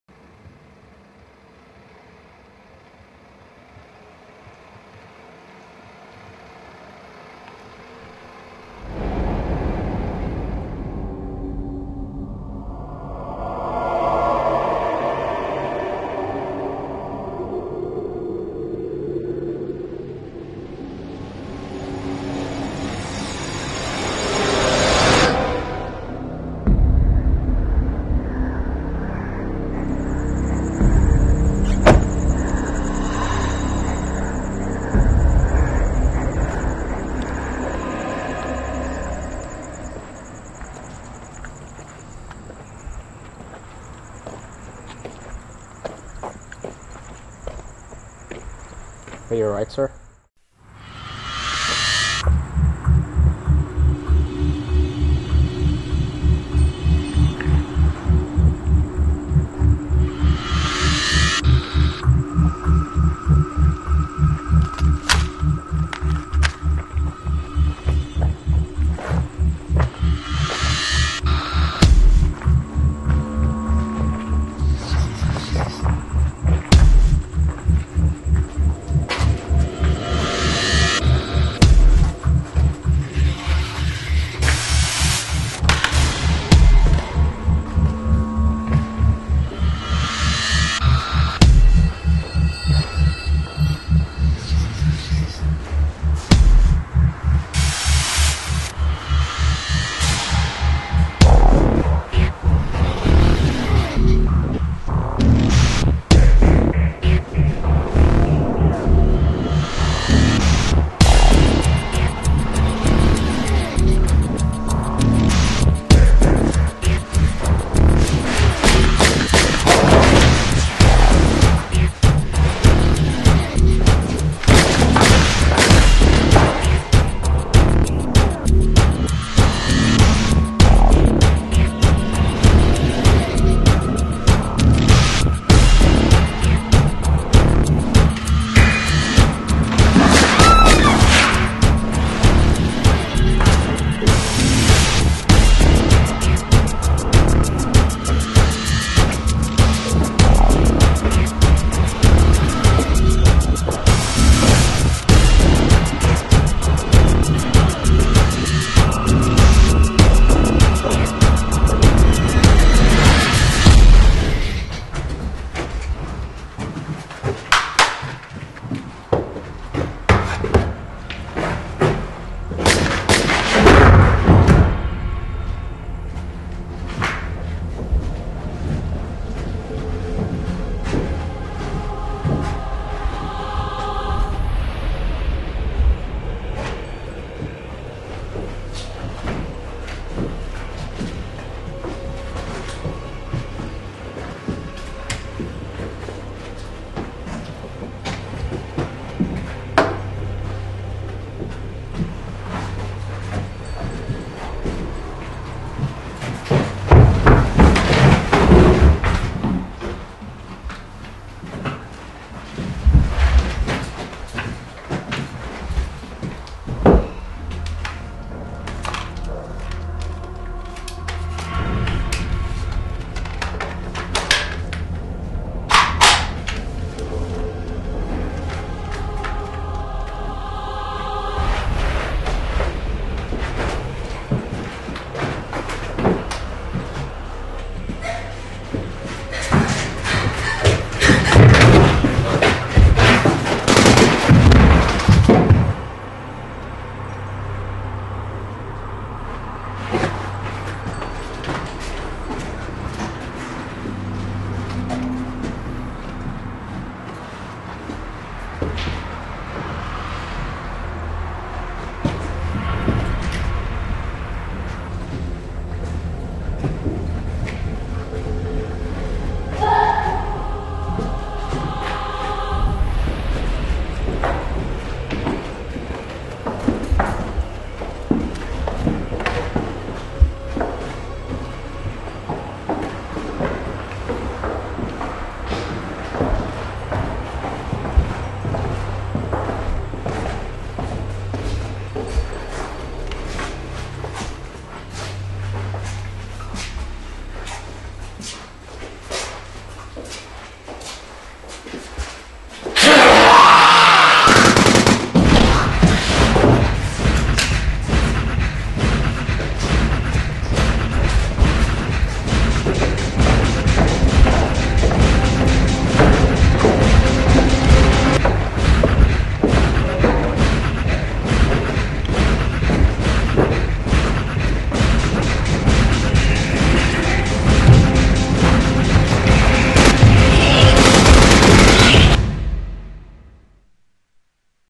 Real life first person shooter
Me and my buddies got together and decided to make a real life first person shooter game and at the end it turned out really amazing!